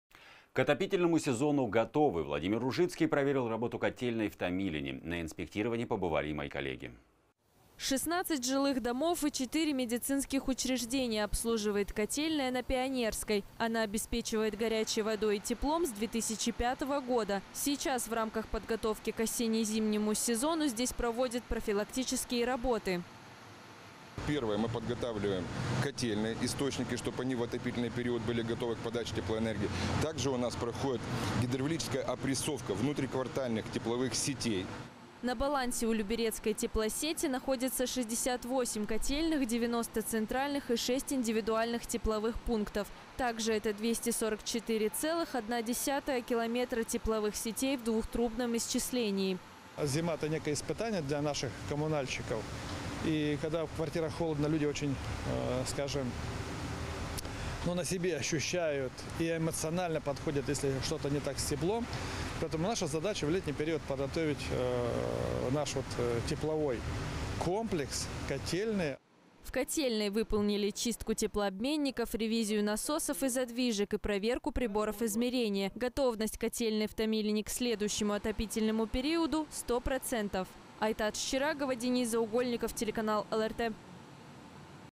Подготовка котельной к осенне - зимнему периоду
К отопительному сезону готовы! Глава Люберец Владимир Ружицкий проверил работу котельной в Томилине. На инспектировании побывала наша съёмочная группа.

16 жилых домов и 4 медицинских учреждения обслуживает котельная на Пионерской. Она обеспечивает горячей водой и теплом с 2005 года. Сейчас в рамках подготовки к осенне-зимнему сезону здесь проводят профилактические работы.

Василий Власов, заместитель главы администрации г.о. Люберцы:
«Первое – мы подготавливаем котельные источники, чтобы они в отопительный период были готовы к подаче теплоэнергии. Также у нас проходит гидравлическая опрессовка внутриквартальных тепловых сетей».

На балансе у Люберецкой теплосети находятся 68 котельных, 90 центральных и 6 индивидуальных тепловых пунктов. Также это 244,1 километра тепловых сетей в двухтрубном исчислении.

Владимир Ружицкий, глава г.о. Люберцы:
«Зима – это некое испытание для наших коммунальщиков. И когда в квартирах холодно, люди очень на себе ощущают и эмоционально подходят, если что-то не так с теплом. Поэтому наша задача в летний период подготовить наш тепловой комплекс, котельные».

В котельной выполнили чистку теплообменников, ревизию насосов и задвижек, и проверку приборов измерения. Готовность котельной в Томилине к следующему отопительному периоду – 100 процентов.